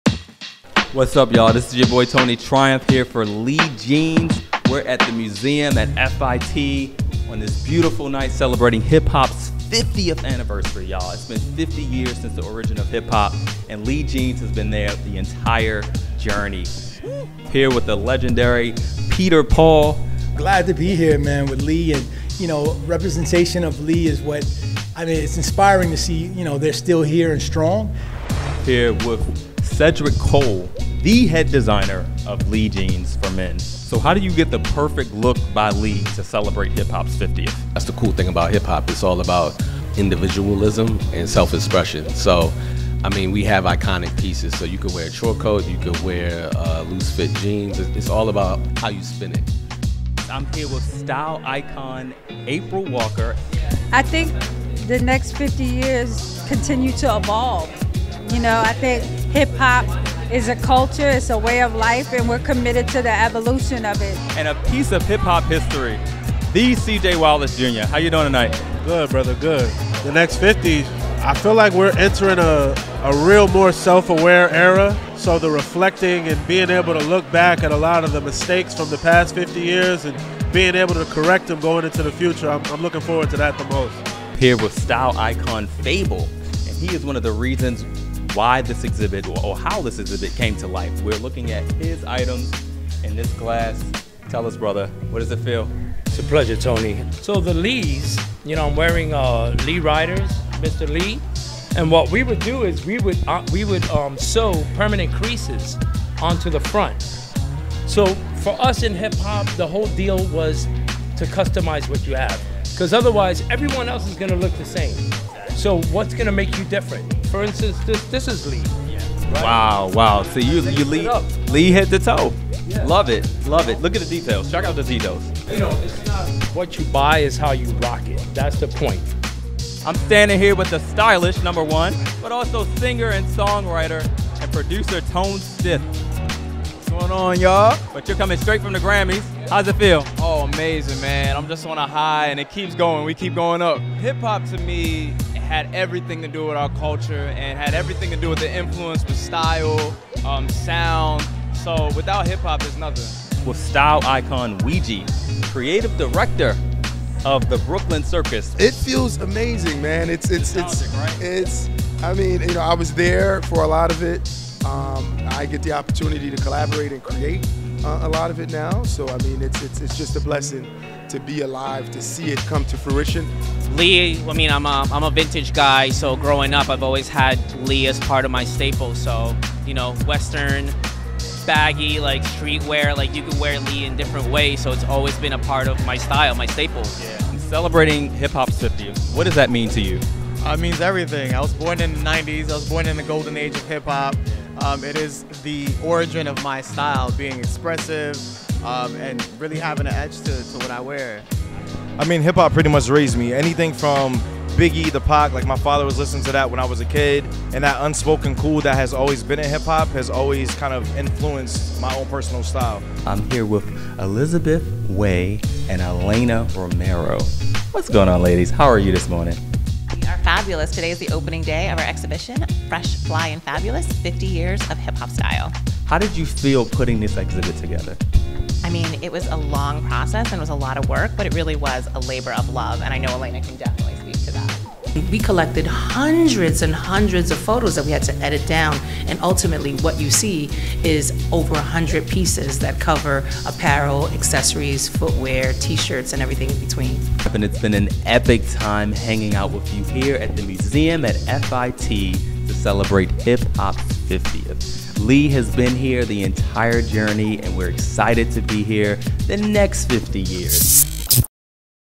Celebrating the Fresh, Fly, and Fabulous: 50 Years of Hip Hop Style Exhibit
Denim and music have always been intertwined as forms of originality and self-expression. And Lee® has been there since the first tremors that triggered the tidal wave known as hip hop, a global force behind today’s fashion and musical world. From the late ‘70s through the ‘80s, Lee jeans and jackets became a uniform of the burgeoning scene surrounding New York.

Fifty years later, hip hop has expanded from a music genre to a global culture. Through this anniversary year, Lee will shine a light on its impact. Hand in hand with Black History Month, Lee sponsored the opening gala of Fresh, Fly, and Fabulous: Fifty Years of Hip Hop Style, an exhibition running from February through April by the Museum at FIT. Hosted by Tony Triumph, hear from exhibit co-curators Elizabeth Way and Elena Romero, Lee’s global director of men’s design, Sedgwick Cole, Jr., and others as they share stories of hip-hop fashion and its influence then and now.